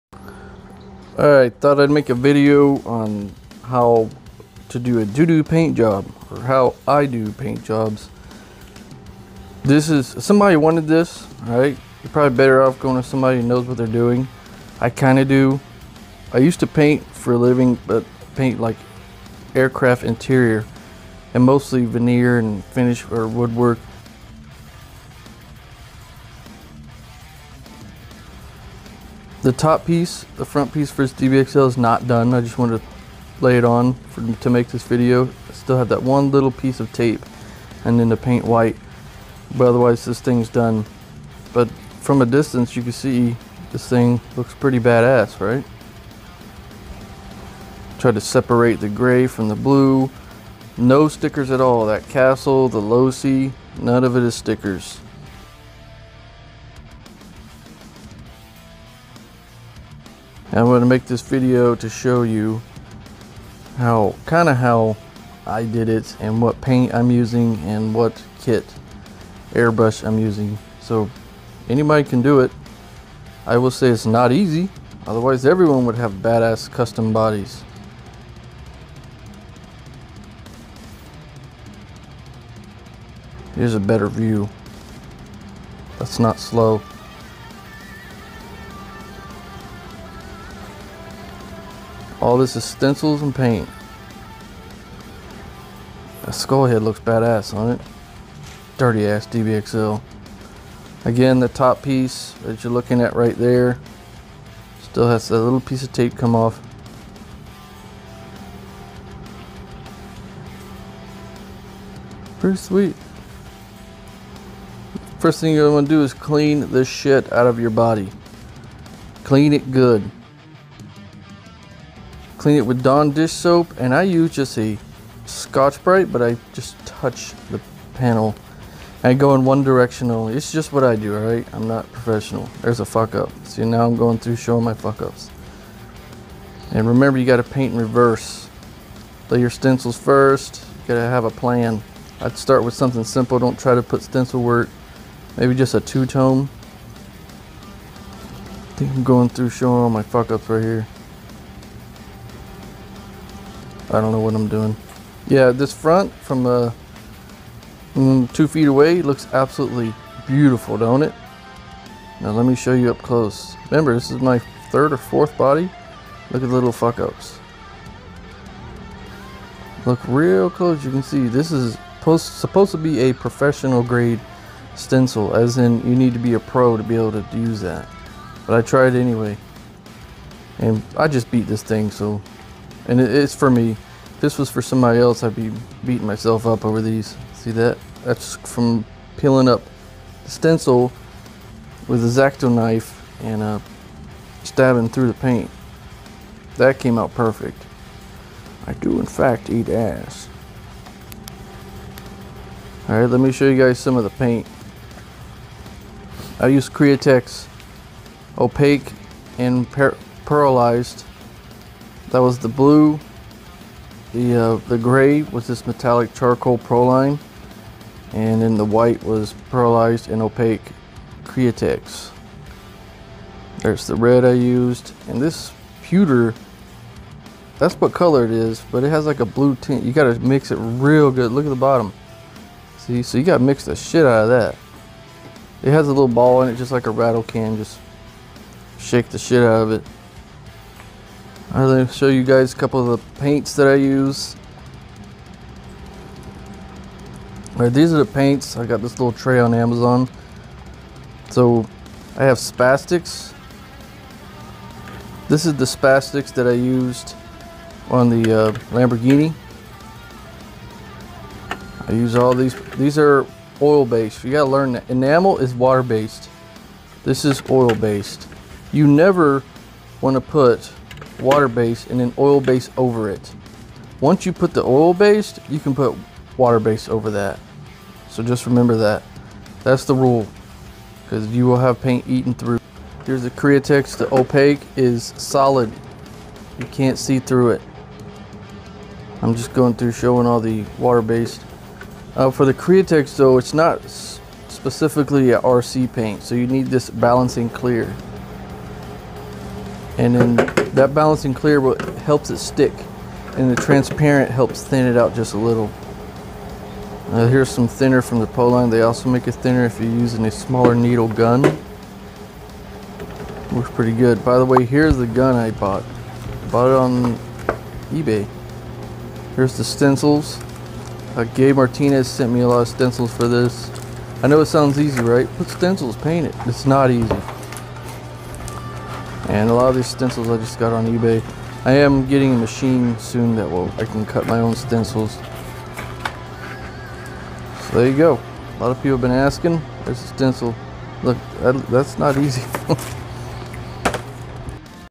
Dbxl-e 2.0 Paint for Dummies
A couple tips I have on painting. Not much but it's what I've learned so far.